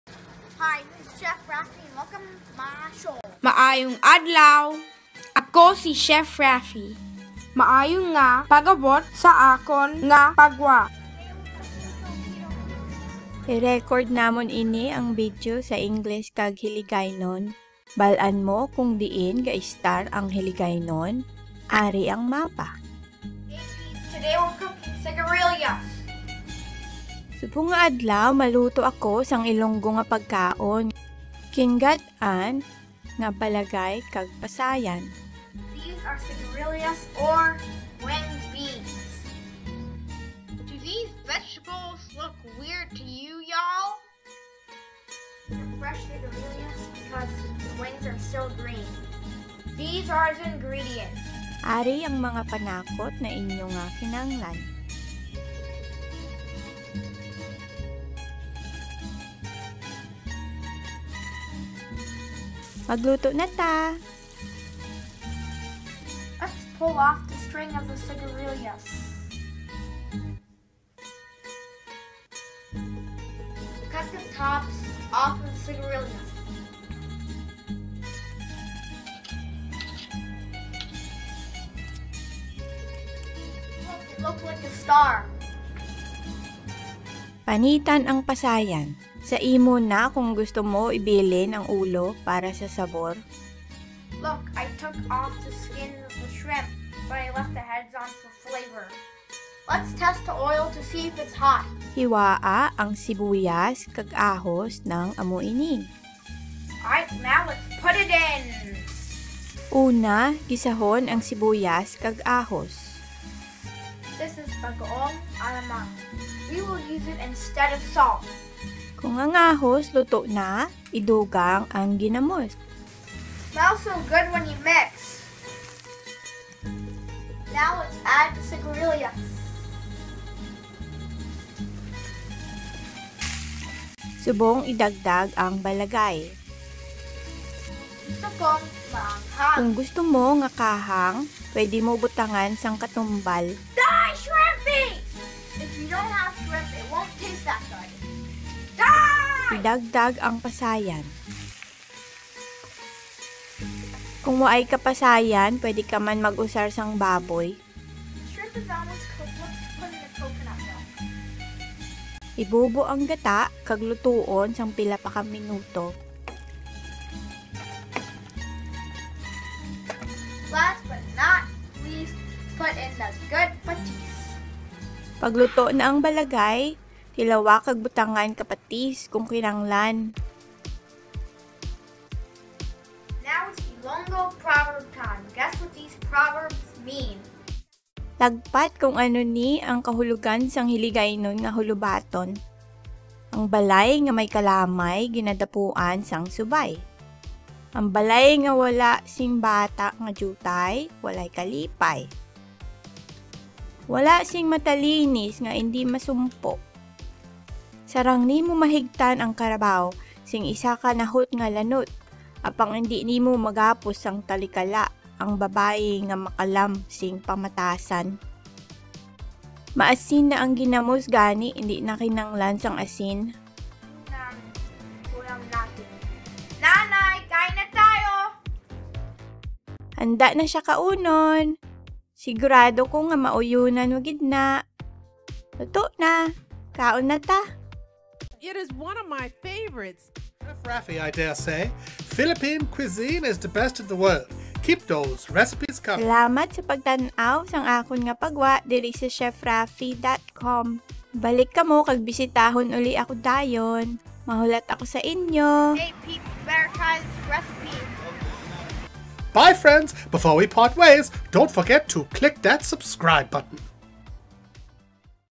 Philippine Cooking: Ginataang Sigarilyas (In Hiligaynon and English)
How to cook winged beans! Called various names throughout the Philippines, Sigarilyas (Tagalog), Pallang (Ilocano), Balagay (Hiligaynon), Winged Bean (English), this unique, tasty vegetable is a must try for all! Learn how to prepare and turn this flavorful vegetable into a delectable meal you will not forget.
   Write us and tell us how you cook sigarilyas or what you call it in your language! We would love to hear from you!
   Chef Rafi will use a recipe from the Southern Philippines and film this video in Hiligaynon Ilonggo and English.

Shopping List:

1 cup coconut milk
1 cup shrimp
2 cups sigarilyas/winged beans
1 tsp shrimp paste (bagoong alamang)
2 cloves garlic
½ onion
1 tsp fish paste/patis (or to taste)
1 tbsp vegetable oil
2 hot chillis/siling labuyo (optional)